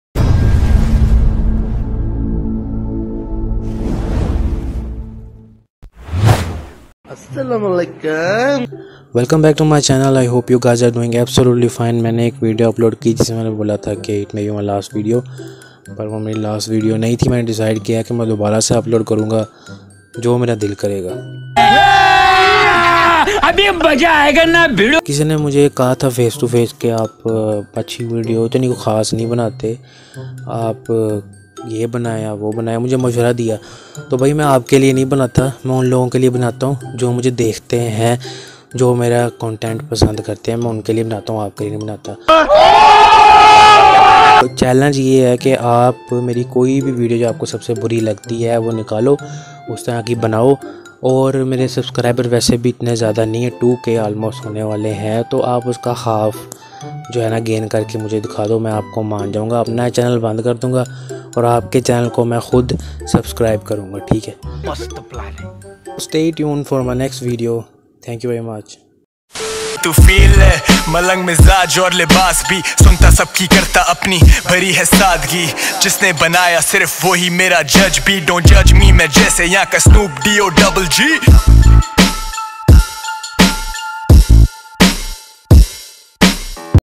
YouTube Return ( @alihassanchughtai7468 )
YouTube Return ( ‎@Ali Hassan Chughtai  )
IN THIS VIDEO I AM RETURNING TO YOUTUBE. AND GAVE A MESSAGE TO HEATERS AND CHALANGE THEM.
I REQUEST TO YOU GUYS FOR SUPPORT